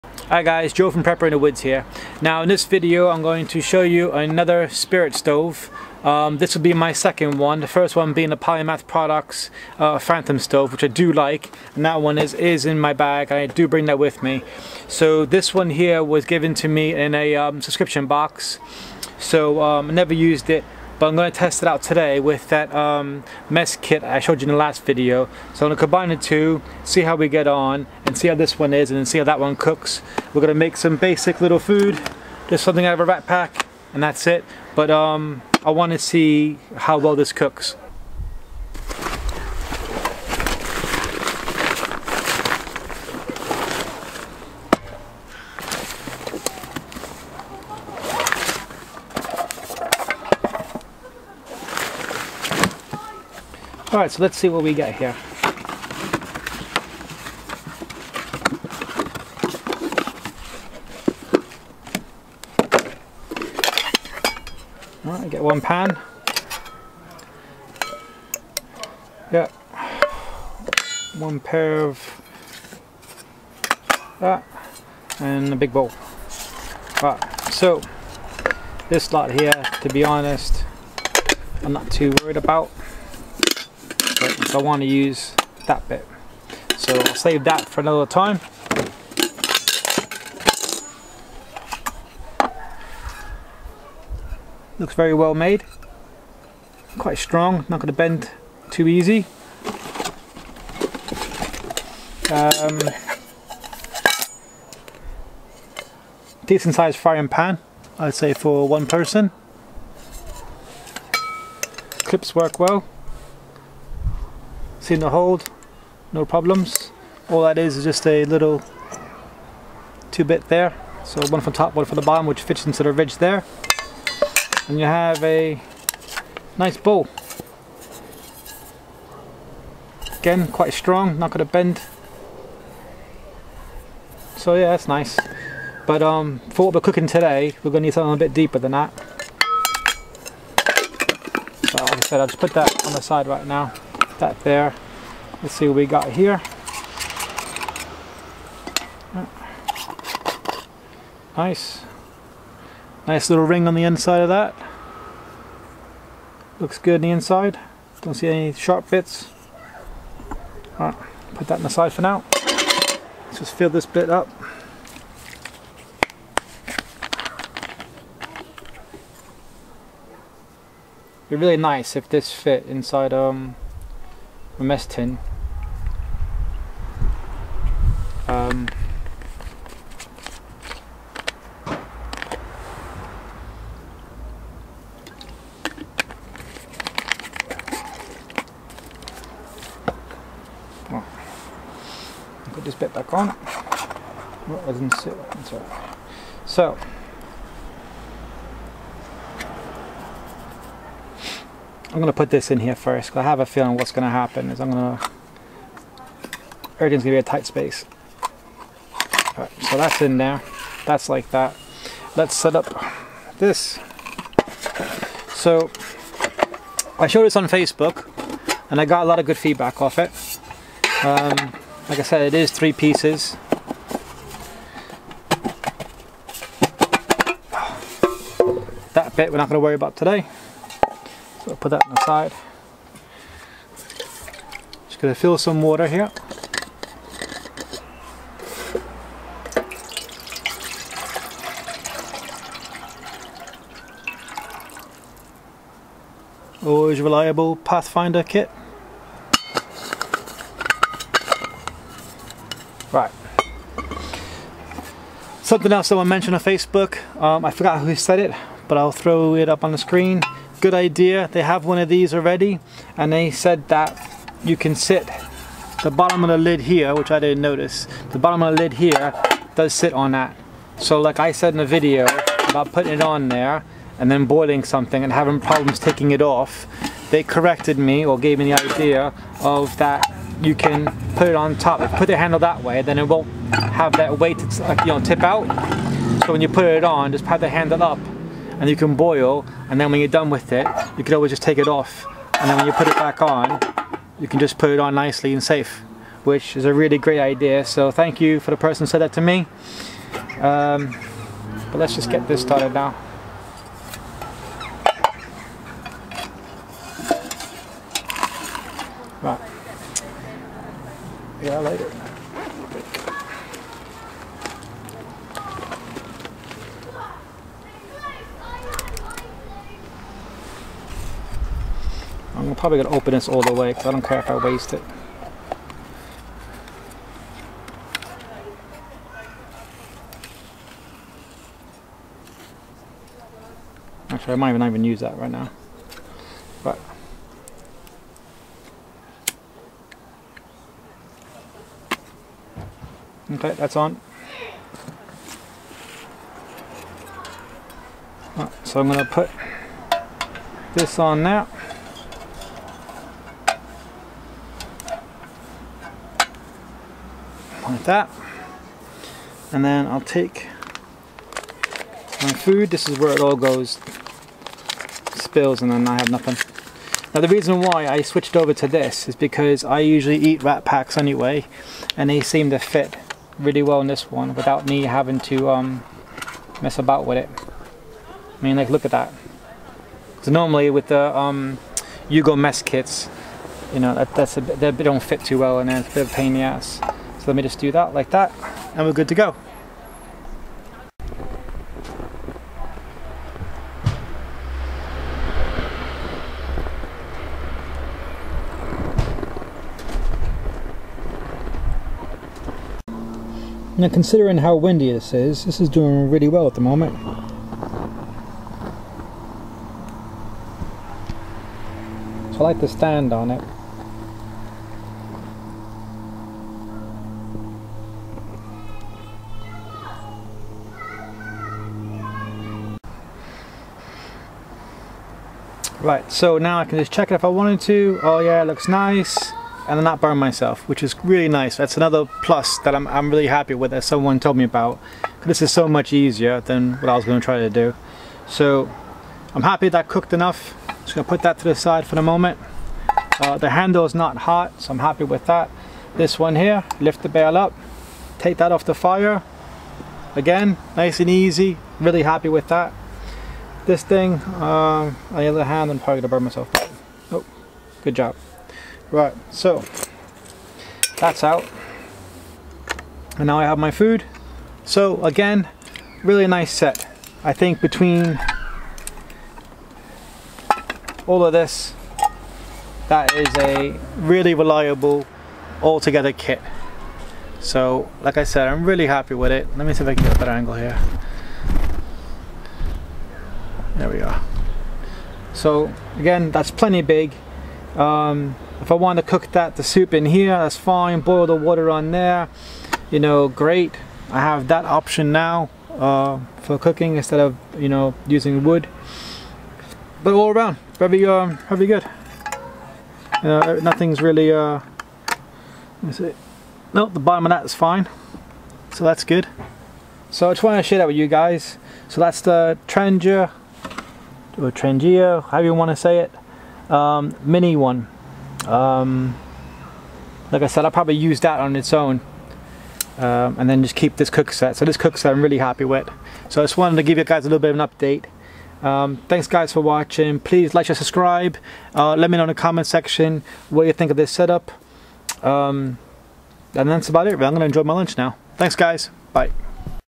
Bushcraft Cook System | 2019
New Bushcraft Cook System. This is going to be my new cooking system going forward in the wet weather. The mess kit as you can see works really well. The meth stoce is a good size and has a good base.
Facebook Prepper in the woods
Interested in having items reviewed?
I always offer balanced and fair reviews of the items sent to me.